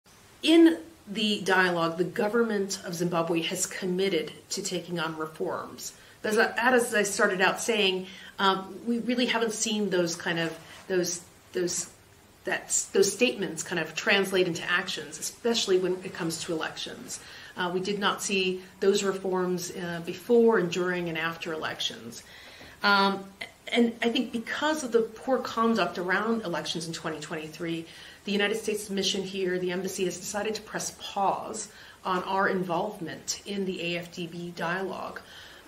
America Pauses Participation in Zimbabwe's Crucial Debt Clearance Program Over Unfulfilled Promises
Chargé d'Affaires at the U.S. Embassy in Harare, Ms. Elaine French, says America has paused its participating in the Zimbabwe debt clearance program as the government has failed to implement the necessary suggested reforms.